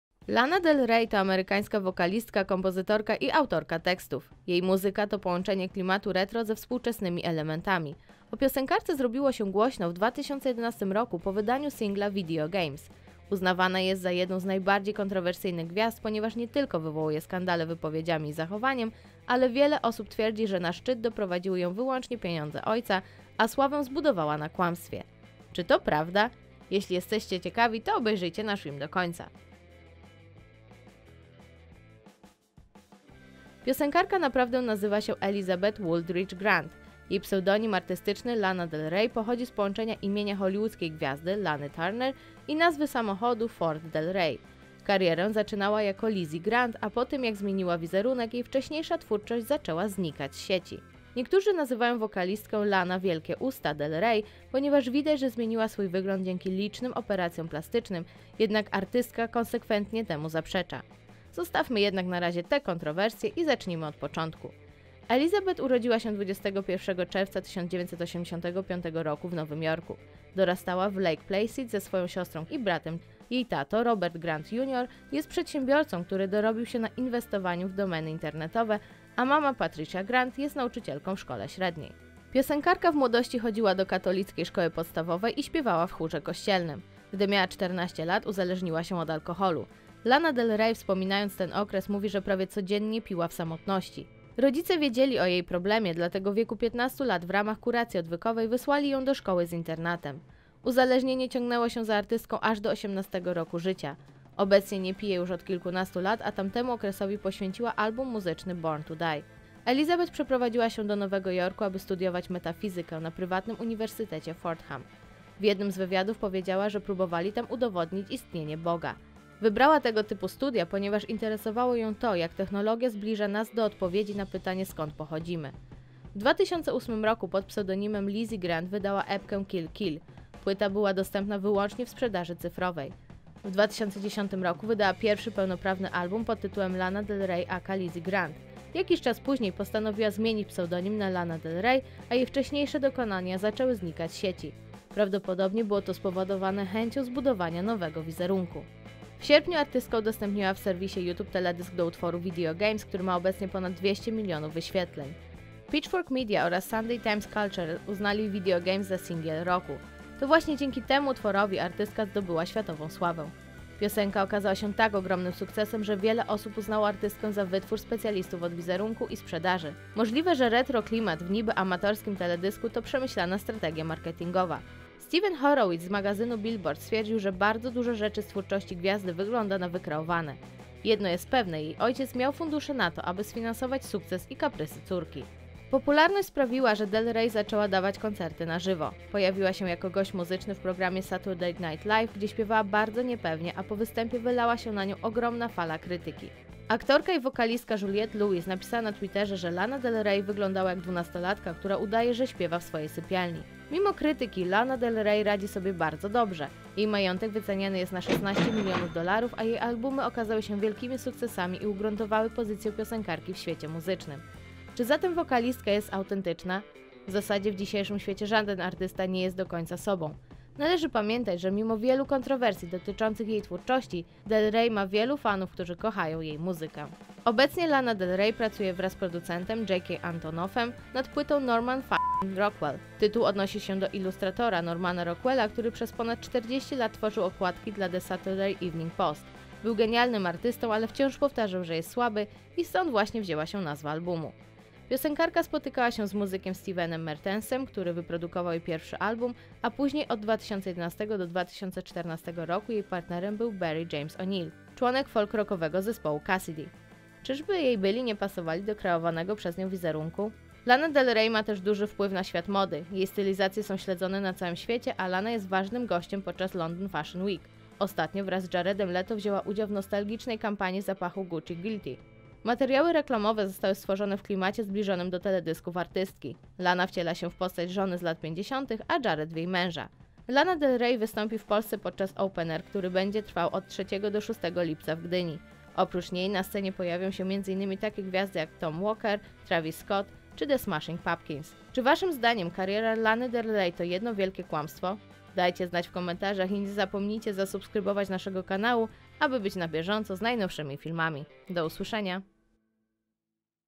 Lana Del Rey - talent czy wytwór marketingu?
Lana Del Rey - talent czy wytwór marketingu? Lana Del Rey wzbudza skrajne opinie. Wielu uważa ją za artystkę wykreowaną przez specjalistów od marketingu i pieniądze ojca, inni za ikonę muzyki i mody. Pewnie część z Was będzie mogła się przekonać jak jest na Open'er Festival, którego piosenkarka będzie gwiazdą!